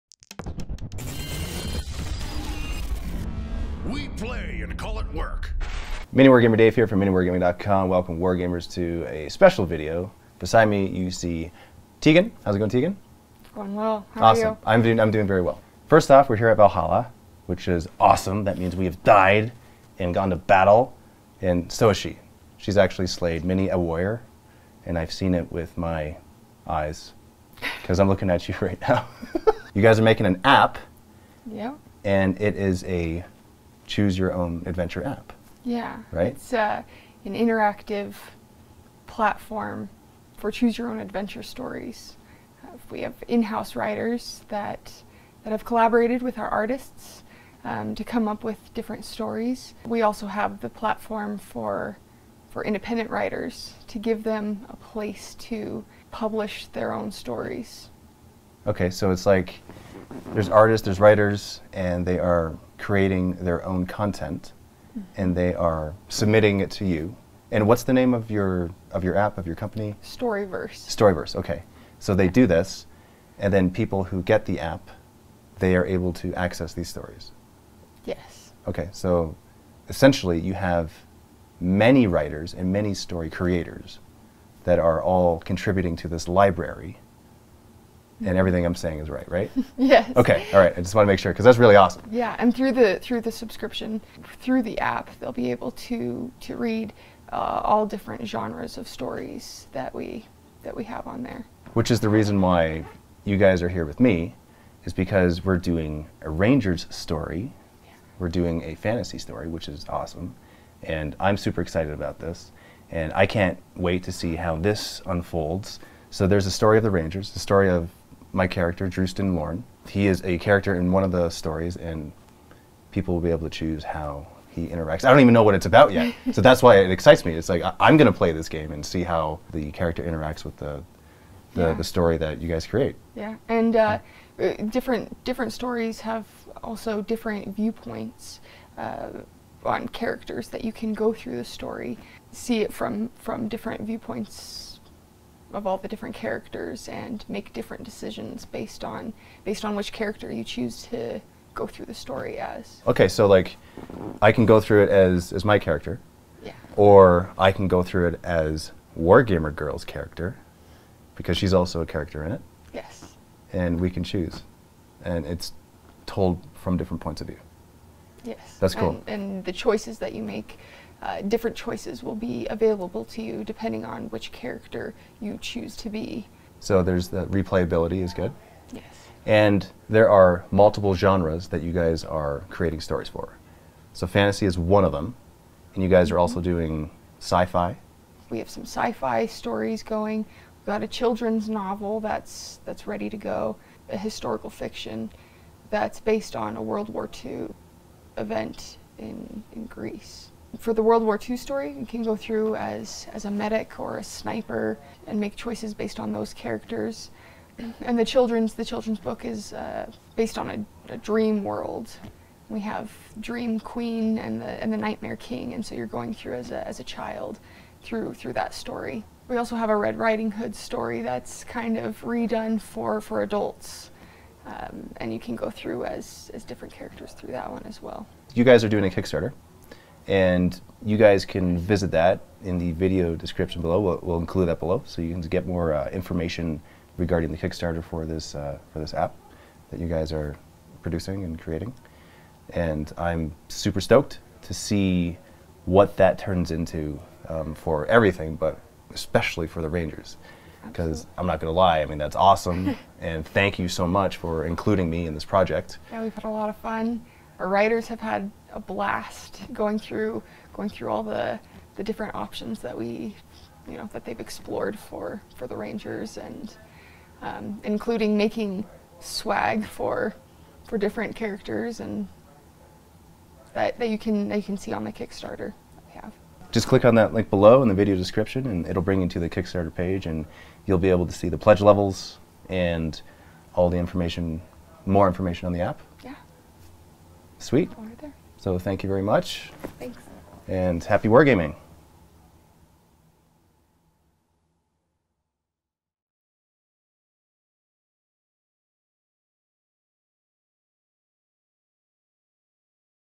Choose Your Own Adventure App - Dave Interview's Storyverse
Dave interviews Teagan from Storyverse: A choose-your-adventure story app currently in development and in the process of becoming a large library of multi-genre stories.